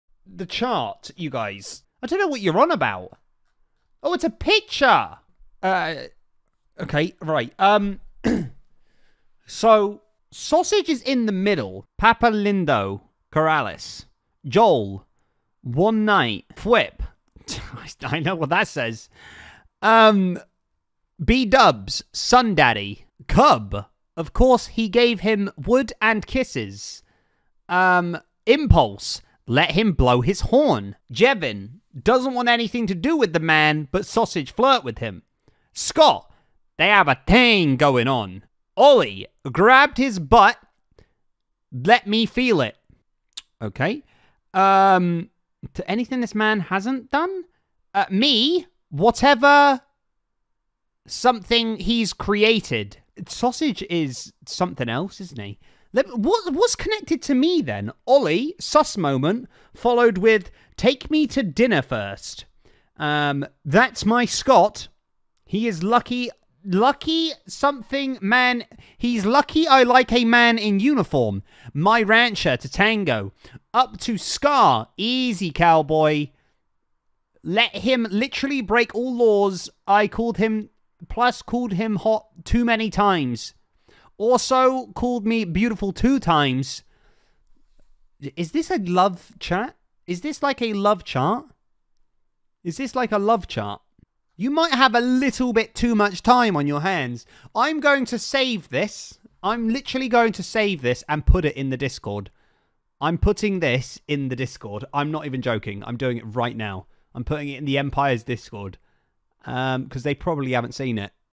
Solidarity REACTS To A EMPIRES SMP x HERMITCRAFT "LOVE" CHART..
Solidarity REACTS To Empires SMP x Hermitcraft "LOVE" chart..